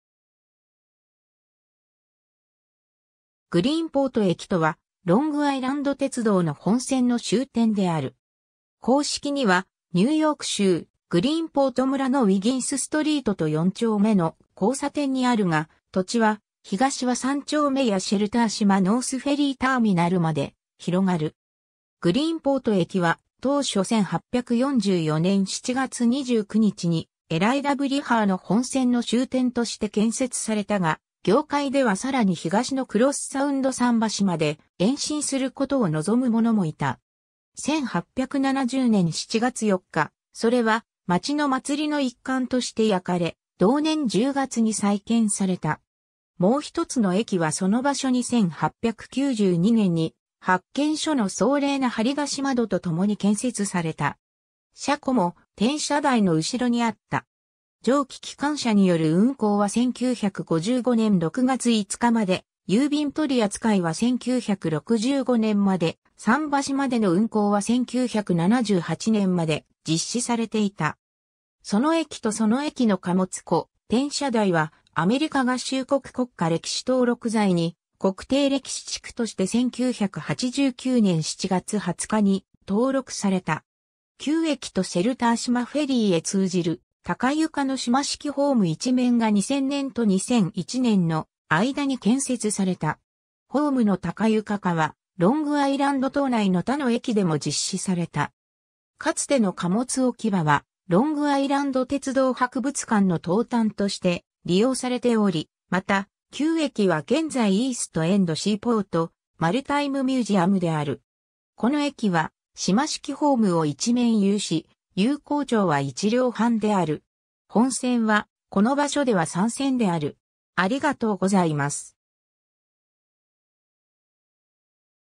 グリーンポート駅
グリーンポート駅（英: Greenport Railroad Station）とはロングアイランド鉄道の本線（グリーンポート支線）の終点である。
公式にはニューヨーク州グリーンポート村のウィギンス・ストリートと4丁目の交差点にあるが、土地 (property) は東は3丁目やシェルター島ノース・フェリー・ターミナルまで広がる。
 グリーンポート駅は当初1844年7月29日にLIRRの本線の終点として建設されたが、業界では更に東のクロスサウンド桟橋まで延伸することを望む者もいた。
1870年7月4日、それは町の祭りの一環として焼かれ、同年10月に再建された。
もう1つの駅はその場所に1892年（一部の文献は1894年と主張）に、発券所の壮麗な張り出し窓（1920年代に撤去された）とともに建設された。
車庫も転車台（石炭置き場に転換された）の後ろにあった。
蒸気機関車による運行は1955年6月5日まで、郵便取り扱いは1965年まで、桟橋までの運行は1978年まで実施されていた。
その駅とその駅の貨物庫 、転車台はアメリカ合衆国国家歴史登録財に国定歴史地区として1989年7月20日に登録された。
旧駅とシェルター島フェリー (Shelter Island Ferry) へ通じる高床の島式ホーム1面が2000年と2001年の間に建設された。
ホームの高床化は、ロングアイランド島内の他の駅でも実施された。
かつての貨物置き場はロングアイランド鉄道博物館の東端として利用されており、また旧駅は現在 East End Seaport Maritime Museum である。
 この駅は島式ホームを1面有し、有効長は1両半である。
本線はこの場所では3線である。